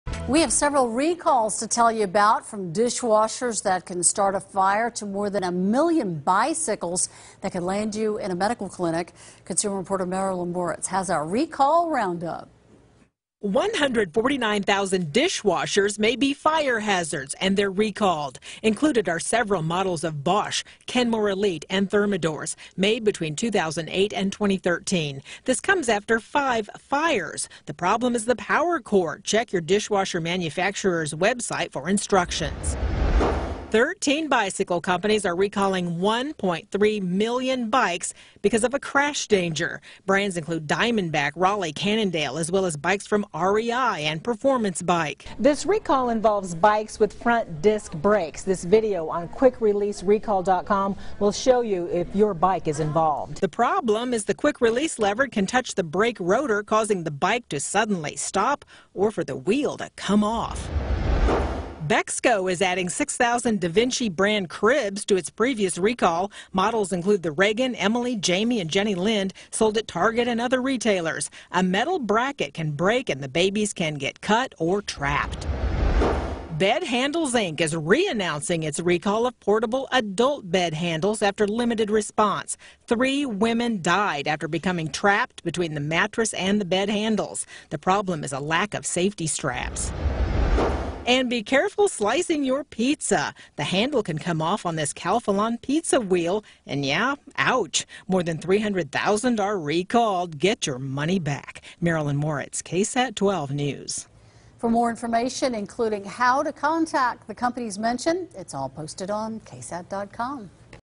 Dishwashers recalled for fire hazard
Nearly 150,000 dishwashers are being recalled because they may start a fire, according to the Consumer Product Safety Commission.